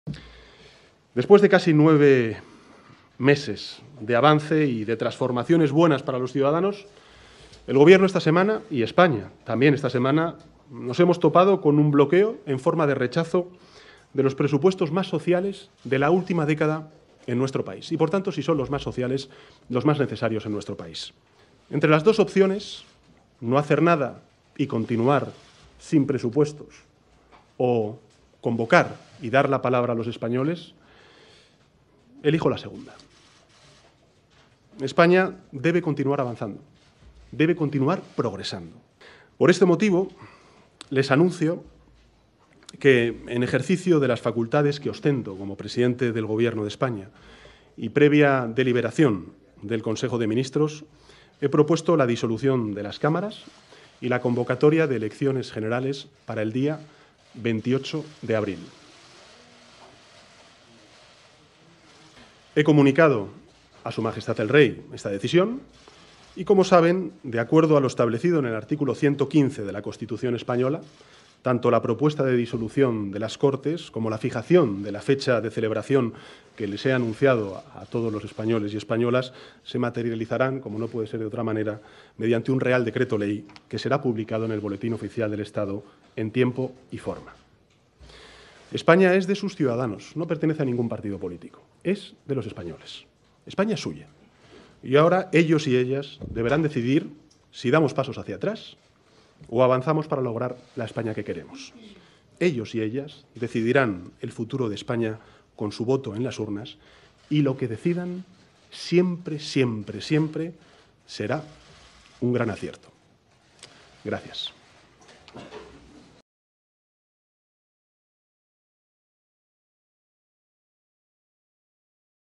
Pedro Sánchez anuncia elecciones generales
España, a elecciones. El presidente del Gobierno, Pedro Sánchez, ha anunciado comicios anticipados para el 28 de abril después del fiasco presupuestario en el Congreso y la ruptura de las negociaciones con la Generalitat de Cataluña.